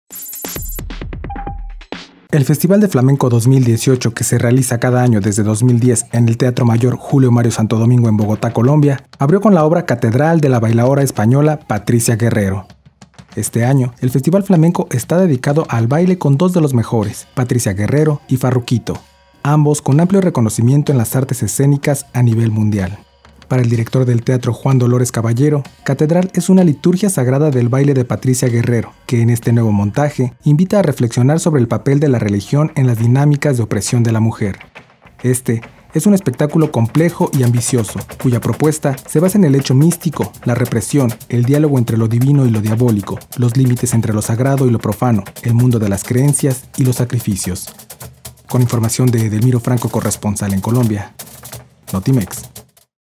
Arranca Festival Flamenco con el espectáculo Catedral
La bailaora Patricia Guerrero abrió El Festival Flamenco 2018 con la obra " Catedral"